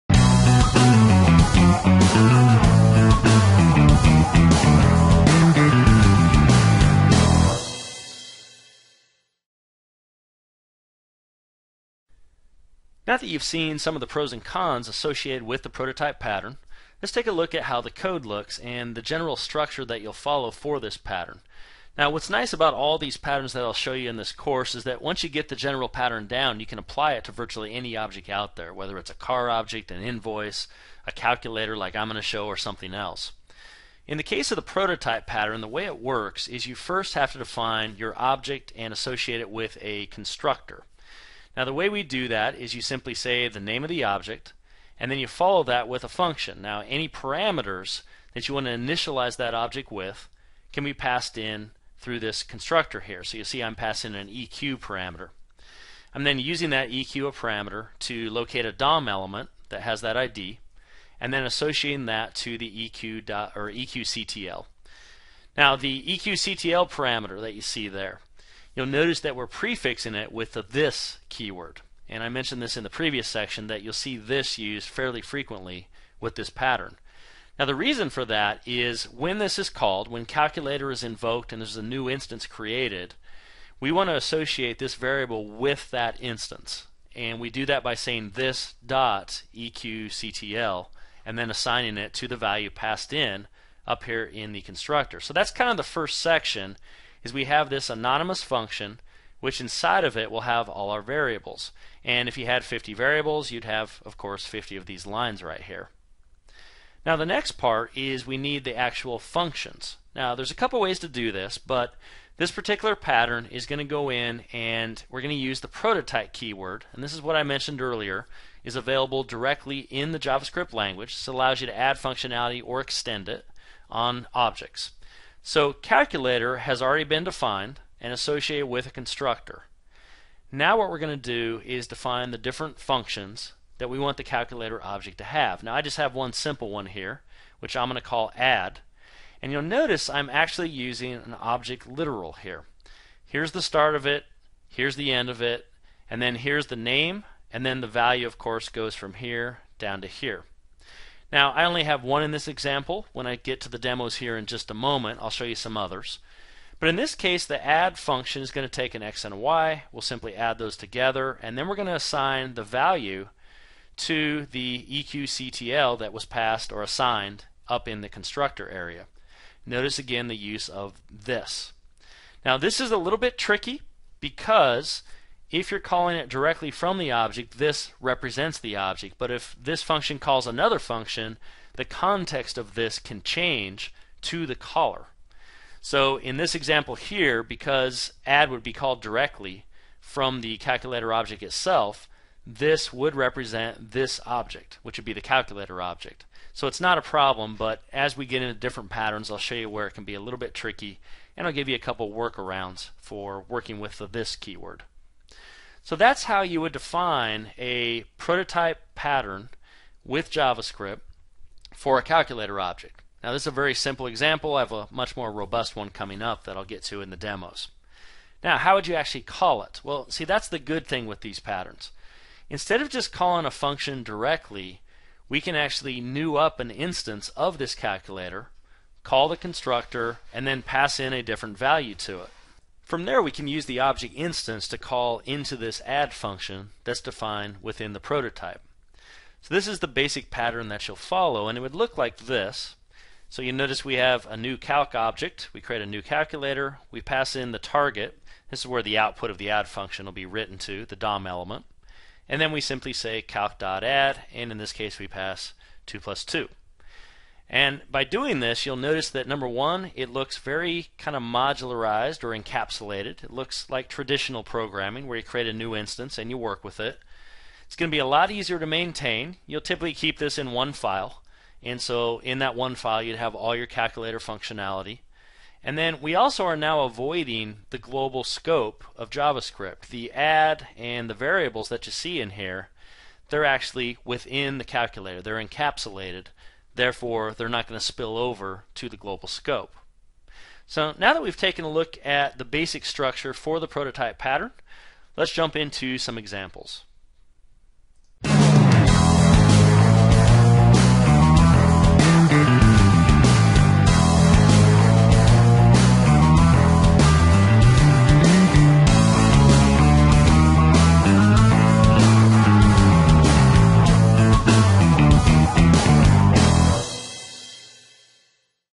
Learn how to stucture Javascript code like a pro | Pluralsight
How do you bring structure to a dynamic language like Javascript?  Dan Wahlin shows you one way to do it using the Prototype pattern in this excerpt from his course "Structuring Javascript Code".  In this video he demos some of the pros and cons of this pattern and how to use it in Javascript.